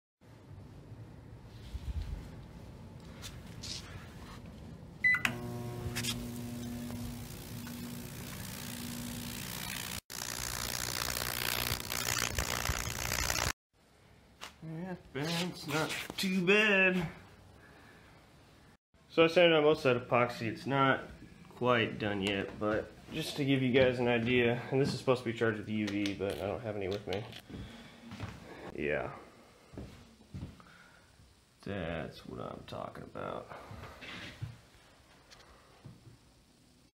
Microwave Projects
I use a microwave transformer to burn wood.